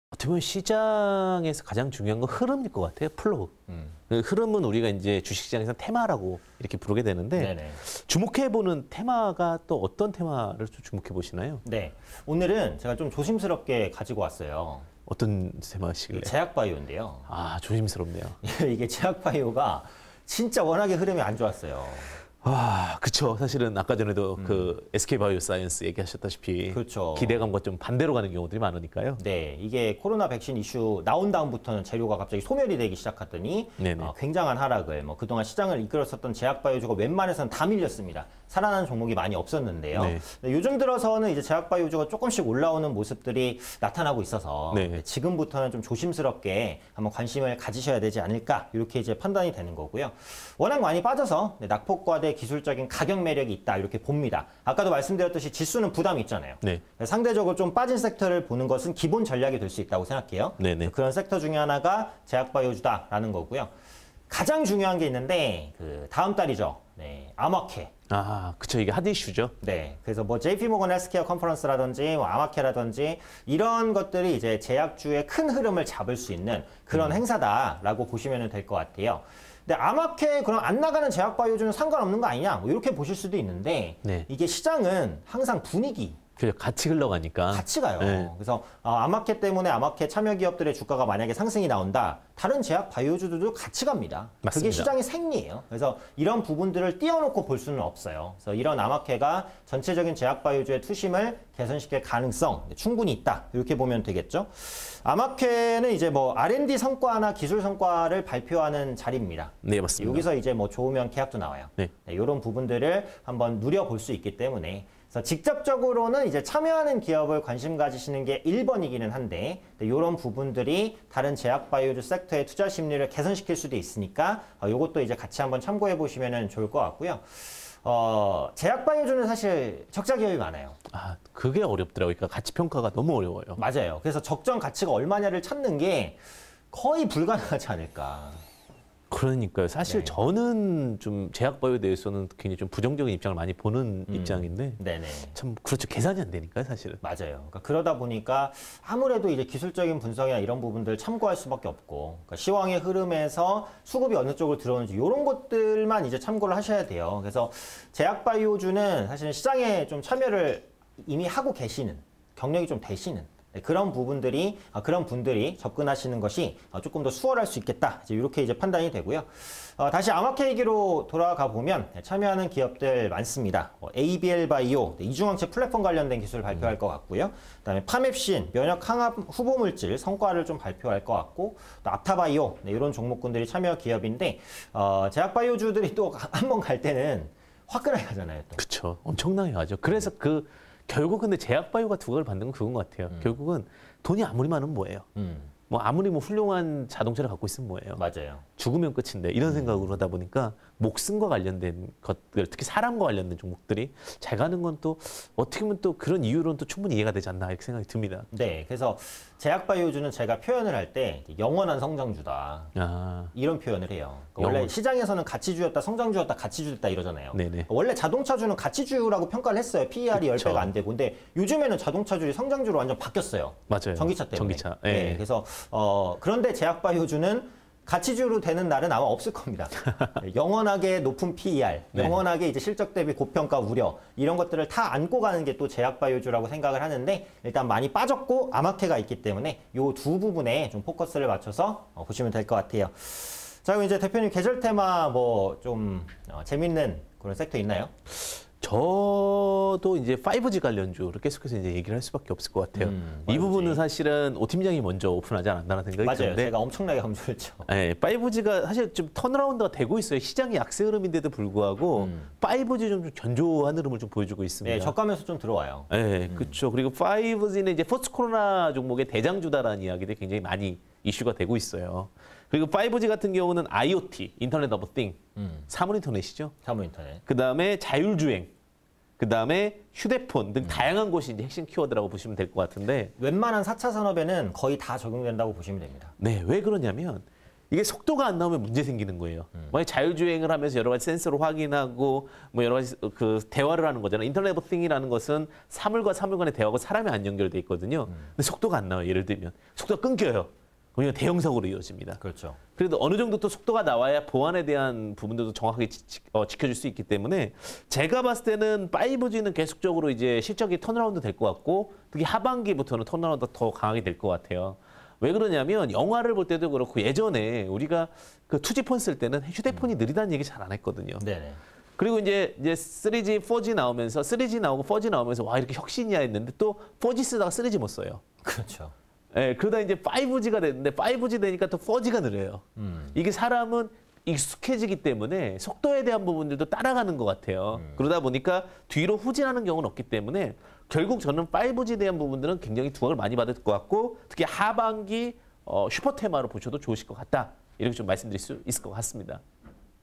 [계절 테마 음식] 제약∙바이오주, 5G 관련주_이데일리맵 황주명&오현진_런앤런 10회 (20210327)
[오현진 - 제약∙바이오 주]
- 코로나 백신 보급 이후 낙폭과대 구간 진입
- 코로나 변이 바이러스 등 코로나19 위협 여전
- 4월 미국암학회(AACR) 예정
- 에이비엘바이오∙파멥신∙압타바이오 관심

[황주명 - 5G 관련주]
-  포스트 코로나 관련주로 두각
-  5G는 IOT, 자율주행, 휴대폰 등 다양한 곳의 핵심 키워드
-  하반기 실적 턴어라운드 기대감